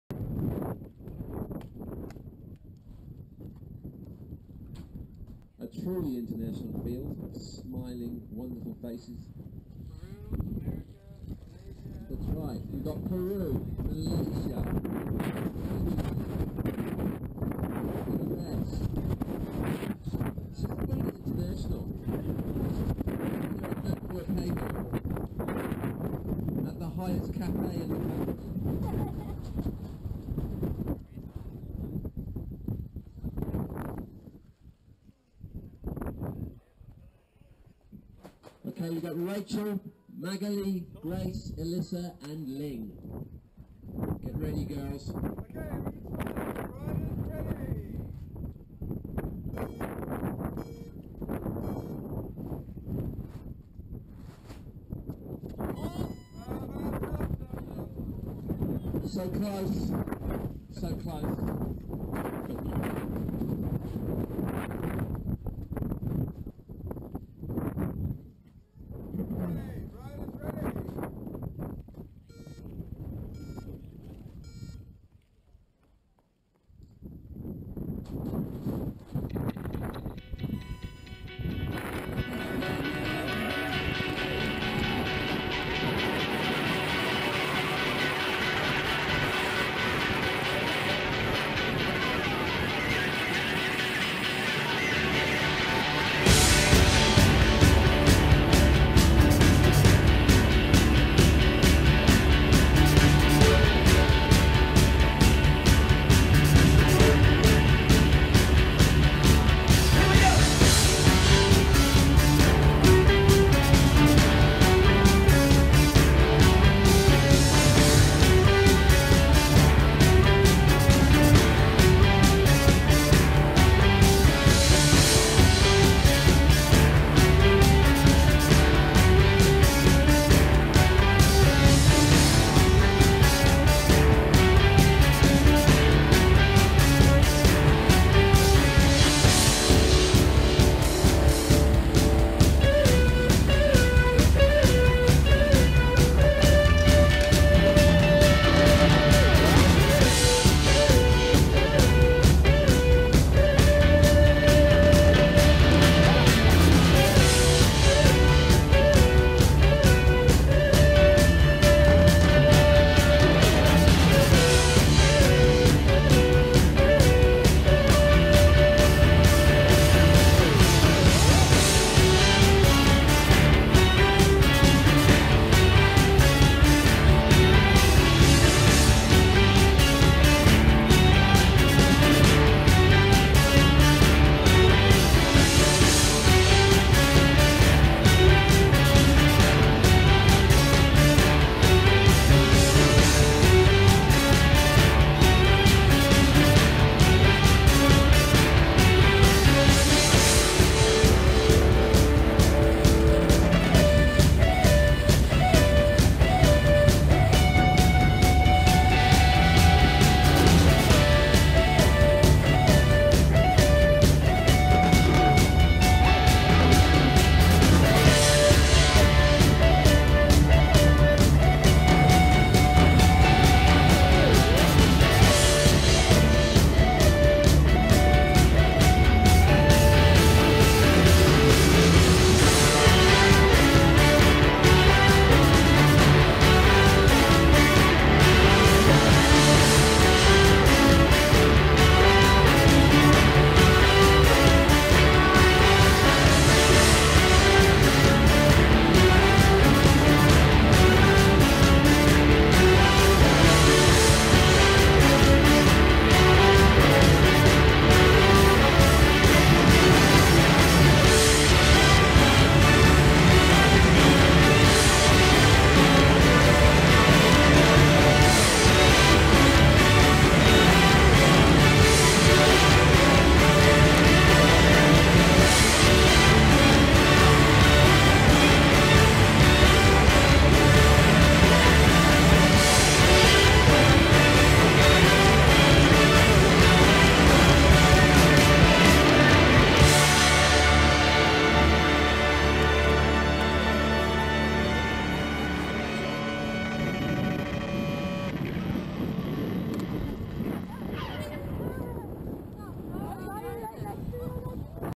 IDF Ruapehu Gravity Festival 2015 - Women's Final
Bagels for the win! Hella stoked to be racing with such a rad girls crew but average footage as I can't keep up with these fast ladies :P

1 Rachel Bruskoff
2 Elissa Mah
3 Magaly Mcwhinnie
4 Ling Lee
5 Grace Wong